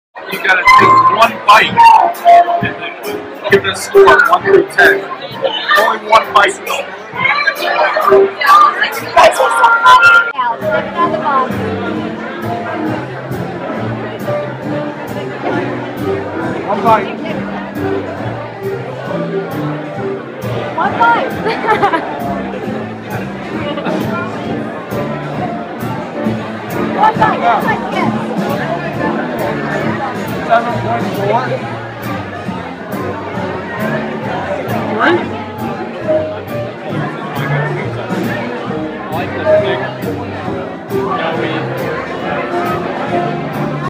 Barstool Pizza Review - Nantucket Pizza With Special Guest Bill Belichick
El Pres meets up with his good friend Bill in Nantucket and Bill does a one bite pizza review.